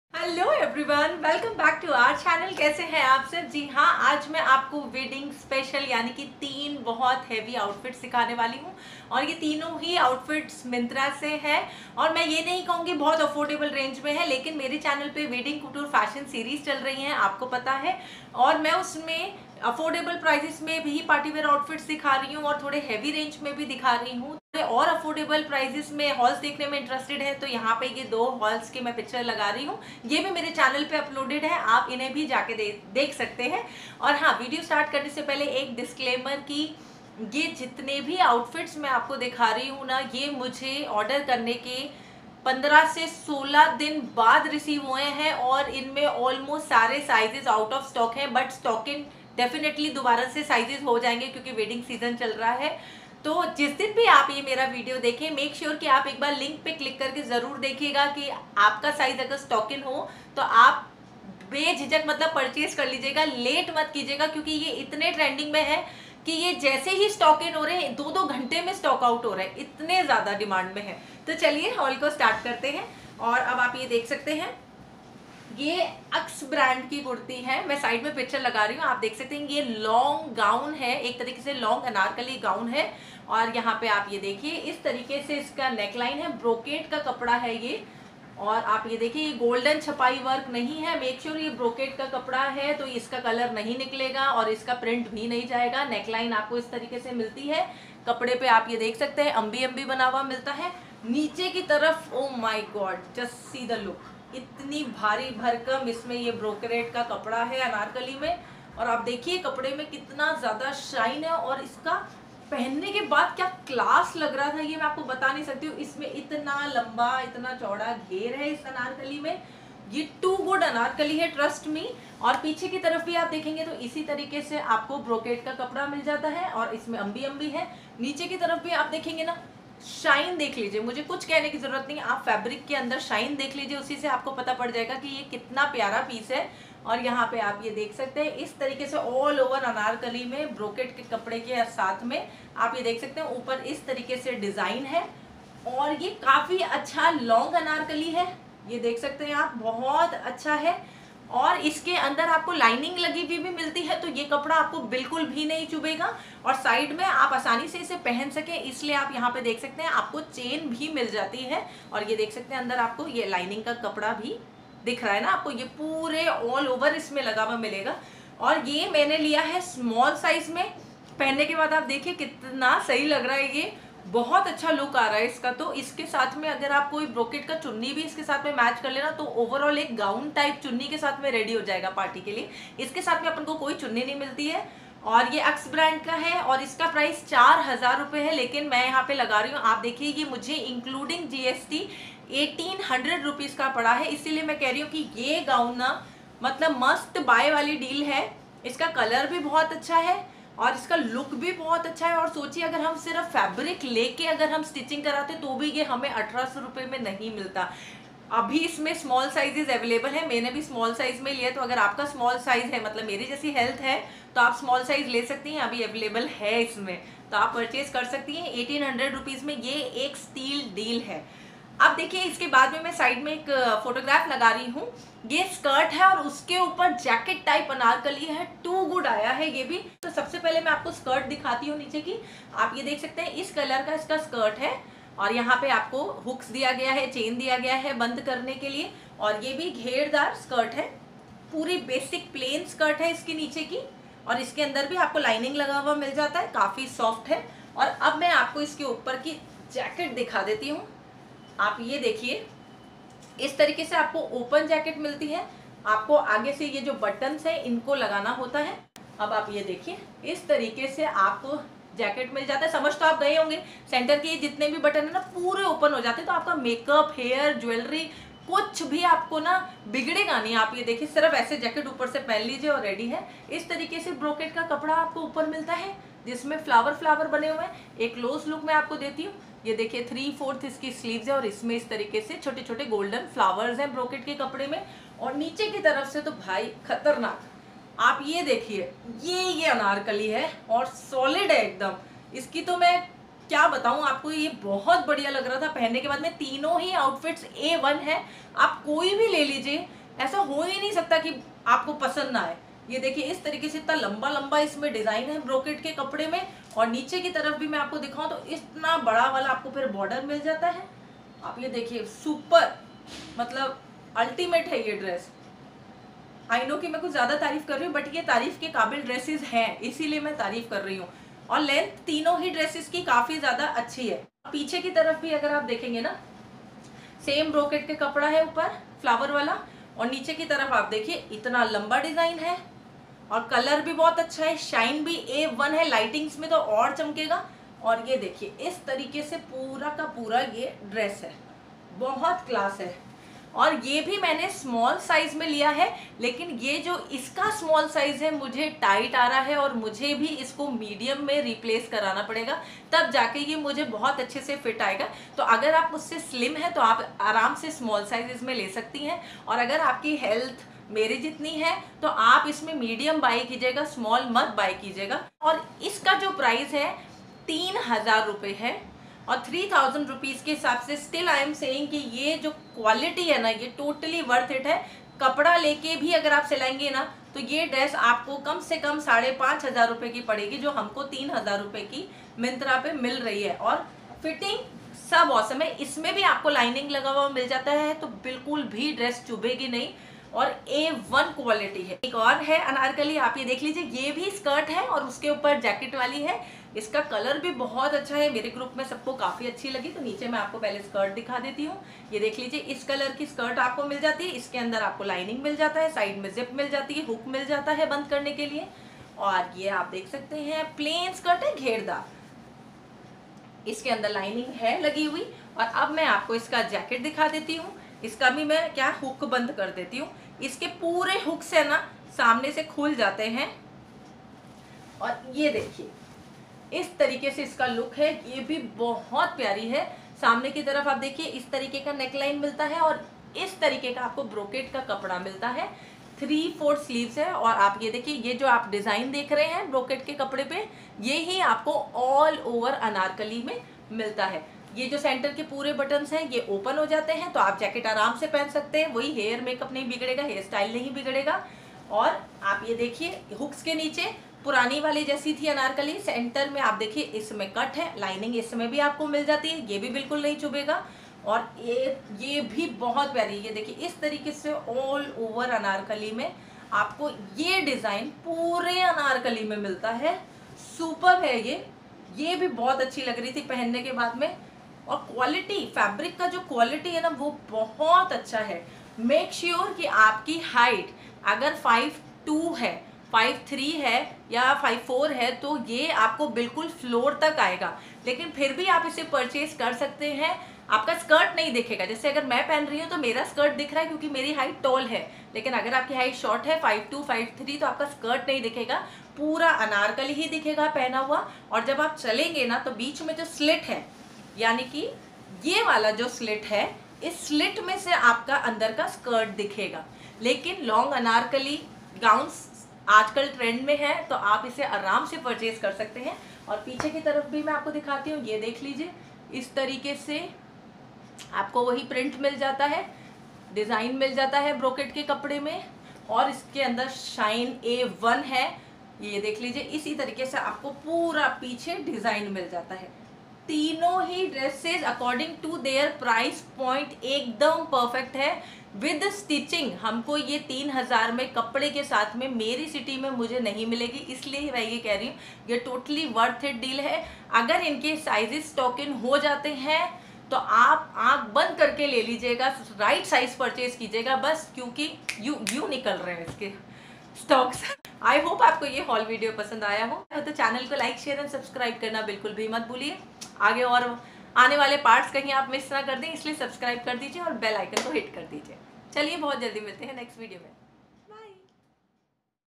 🌟Latest Myntra Kurti Haul 🌟Anarkali Kurtis🌟wedding special outfits(part-3) 🌟preena🌟
hello everyone 😍
today I'm going to Review myntra long anarkali partywear kurtis and the product quality is really really good.